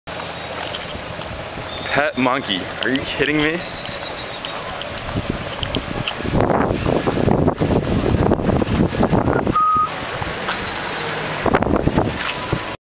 Pet Monkey!
A woman and her pet monkey on a leash in Nicaragua